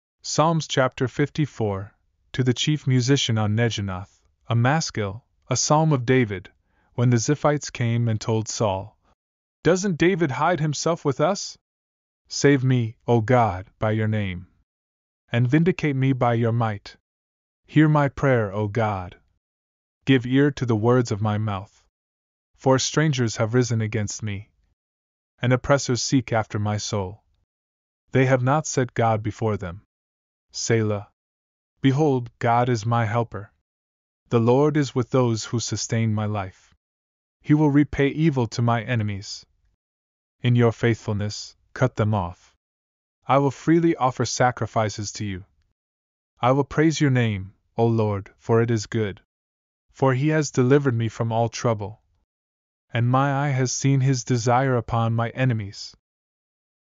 Psalms Chapter 54 - The Holy Bible (KJV) "audiobook - read along" ✝️
Psalms Chapter 54 of 150 The Holy Bible (KJV)

Psalms Chapter 54: (To the Chief Musician on Neginoth, a Maschil, A Psalm of David, when the Ziphites came and told Saul, "Doesn't David hide himself with us?") Save me, O God, by your name, And vindicate me by your might. Hear my prayer, O God; Give ear to the words of my mouth. For strangers have risen against me, And oppressors seek after my soul; They have not set God before them. Selah. Behold, God is my helper; The Lord is with those who sustain my life. He will repay evil to my enemies; In your faithfulness, cut them off. I will freely offer sacrifices to you; I will praise your name, O LORD, for it is good. For He has delivered me from all trouble, And my eye has seen His desire upon my enemies.